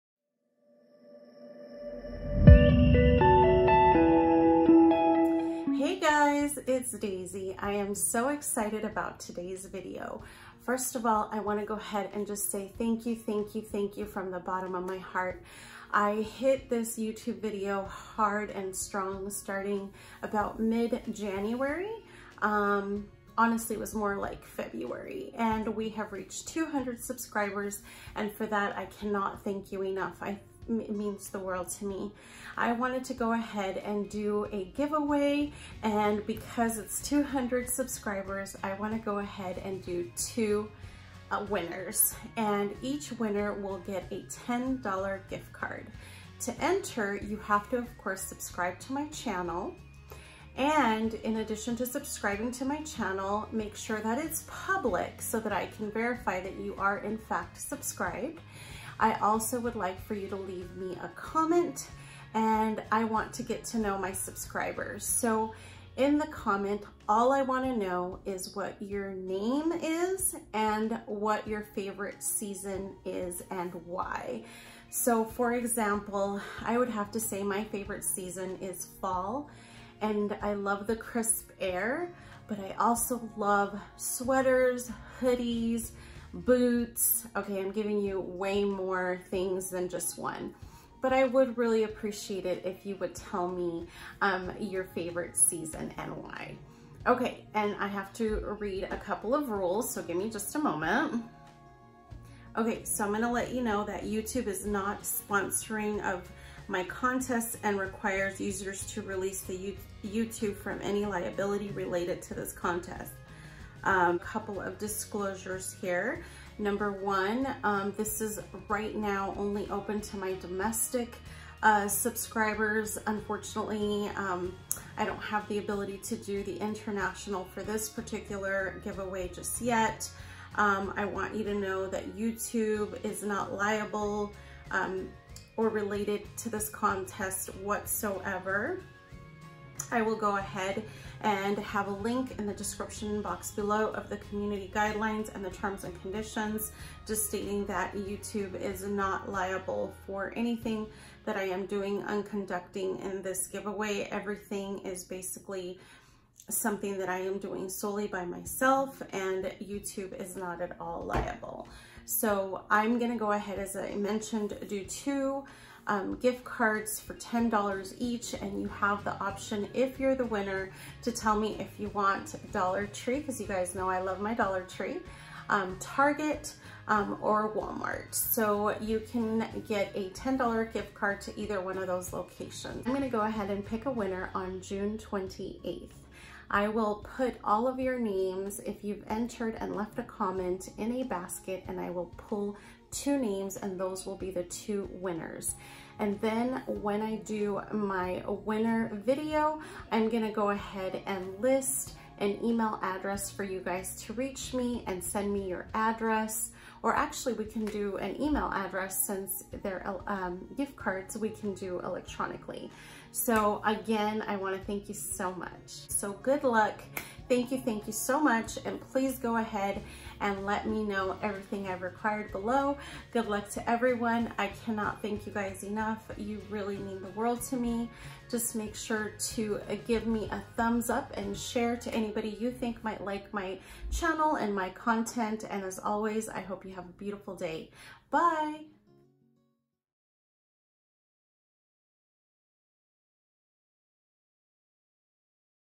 200 Subscriber GIVEAWAY!!! ***CLOSED***
I cannot begin to thank you all enough, I am so happy to have reached this milestone! THANK YOU SO SO MUCH!

You will be two winner of $10 gift cards at either
Dollar Tree
Target or
Walmart

***This giveaway is open to our domestic US subscribers, and all entrants must be 18 years of age (or older) to participate, or have a parent enter for you, and we will be verifying this. The contest ends on Friday night, June 28 at 5:30pm MT, and the winner will be announced the day after the contest ends (June 28th), via
New video Announcement.

Any entries not following the YouTube Community Guidelines will be disqualified. Personal data from the giveaway will NOT be used or shared in any way, shape or form.